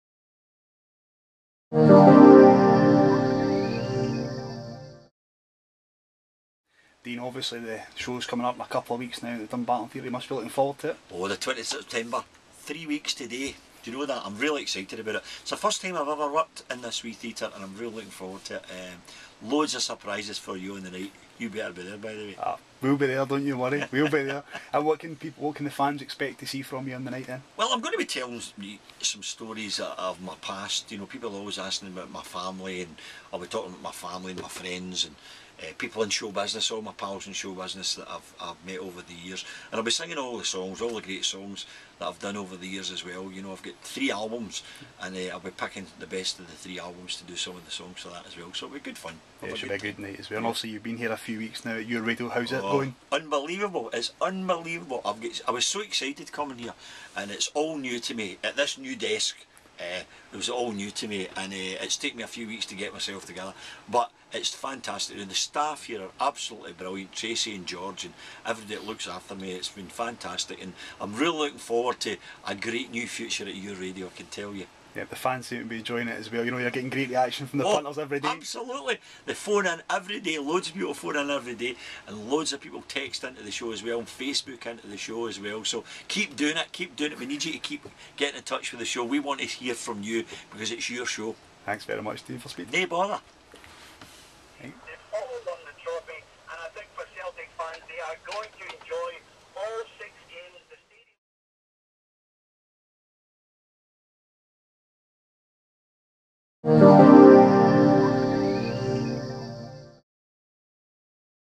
Dean Park (HA)
We took the chance to catch up with Your Radio DJ Dean Park, as he prepares to perform in the Denny Civic Theatre in Dumbarton on Friday, September 20.